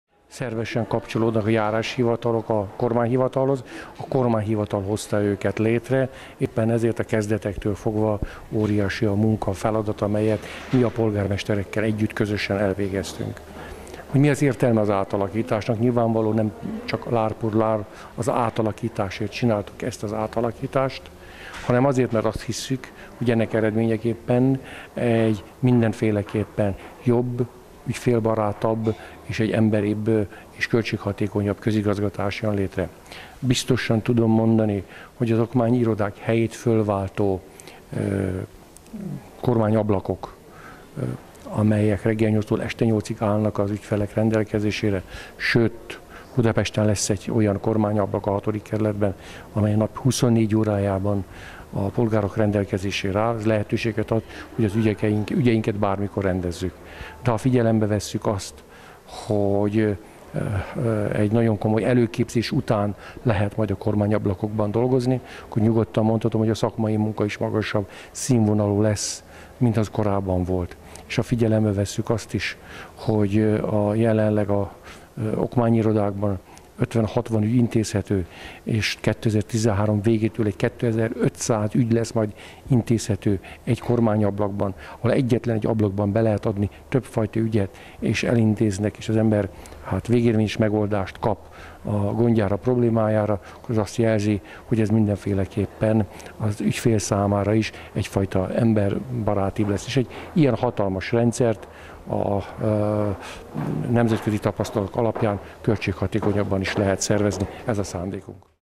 Dr. Pesti Imre interjú- Aláírták a járási megállapodásokat 2012. 10. 30.
Dr. Pesti Imre kormánymegbízott Budapest Főváros Kormányhivatala